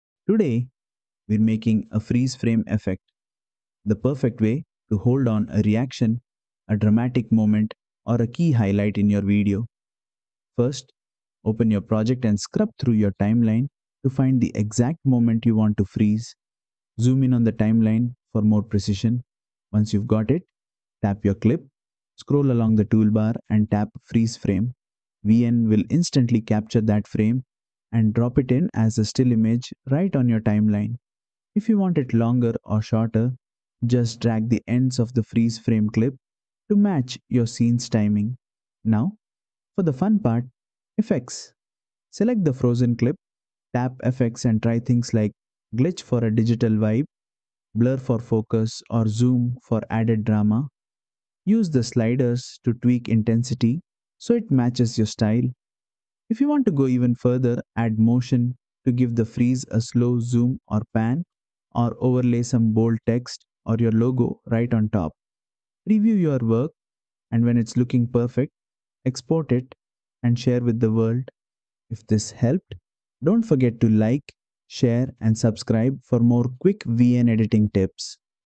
How to Add Freeze Frame Effect in VN Video Editor Mobile - Lesson 29 VN Video Editor Tutorial
In this VN Video Editor tutorial, I’ll show you how to create a Freeze Frame effect — perfect for adding emphasis, dramatic pauses, or stylish text overlays in your video.

We’ll cover:

How to find the exact frame to freeze

Using VN’s Freeze Frame tool

Extending the frozen image duration

Adding extra effects to your freeze frame

By the end, you’ll be able to pause action at just the right moment for maximum impact.

Steps to Make a Freeze Frame Effect in VN Video Editor:

Open Your Project

Load the project with the clip you want to freeze.

Find the Perfect Frame

Move the playhead along the timeline until you land on the exact frame you want to pause.

Tap Freeze Frame

In the bottom toolbar, scroll until you see Freeze Frame and tap it.

VN Splits and Inserts a Still Image

VN will automatically create a still image of that exact frame and insert it into the timeline at the playhead position.

Adjust Freeze Duration

Tap on the frozen frame clip and drag its edges to make it longer or shorter depending on how long you want the pause.

Optional – Add Text or Effects

While the frame is frozen, you can overlay text, stickers, or filters to make the moment more dynamic.

Preview Your Effect

Tap Play to make sure the freeze looks natural and fits your timing.